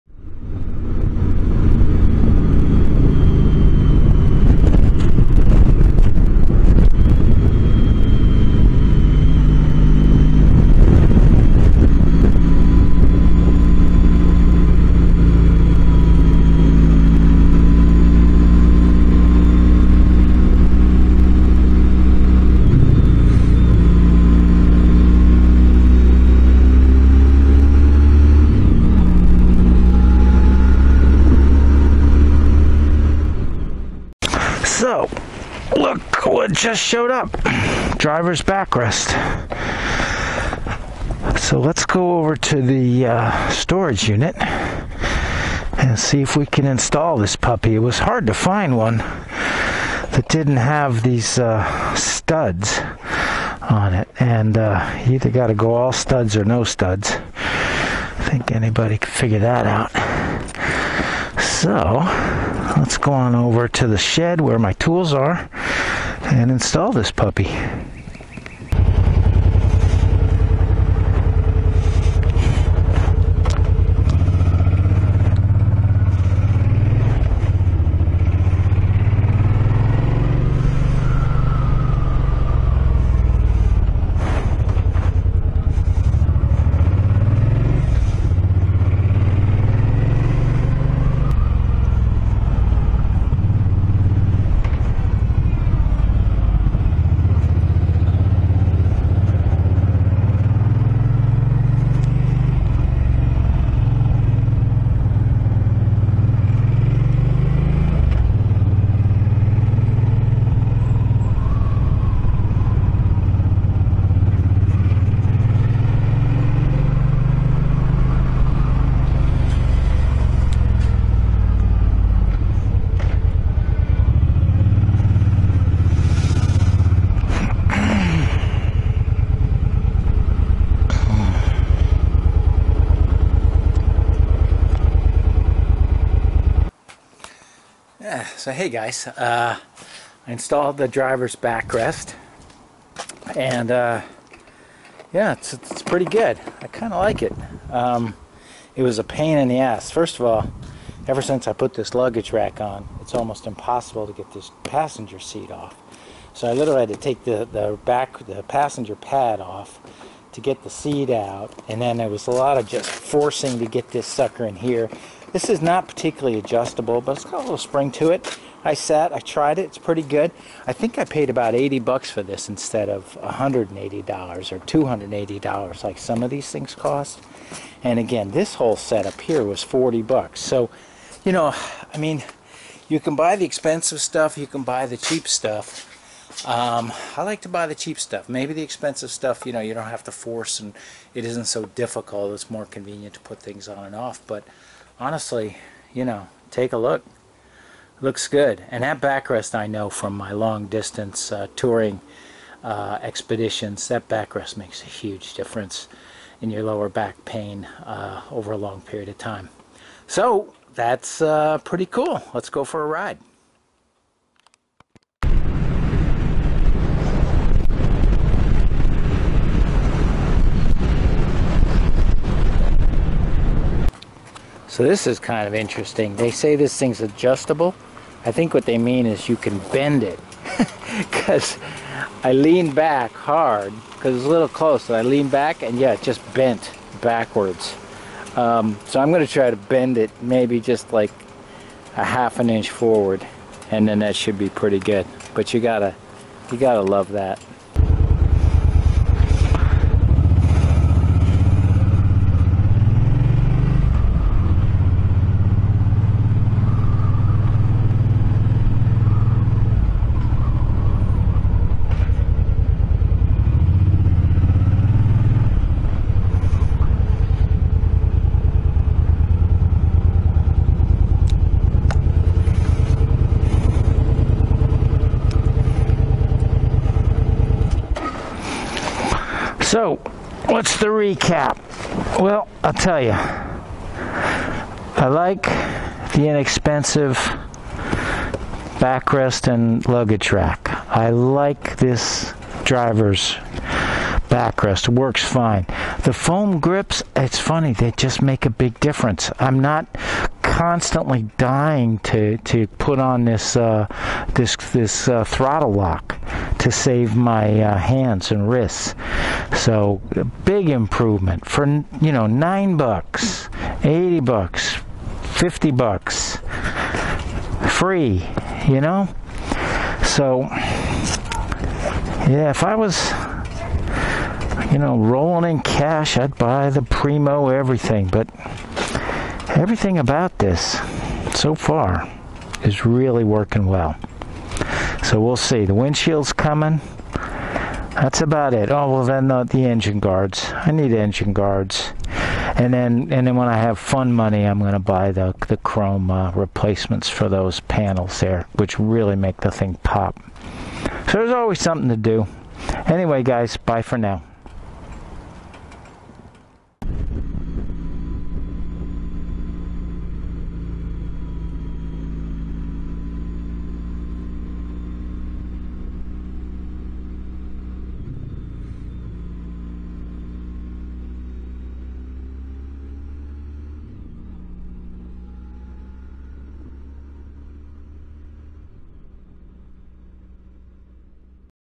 Suzuki C50T: New Driver's Backrest
New affordable driver's backrest for my Suzuki Boulevard C50T.

Here's where I got it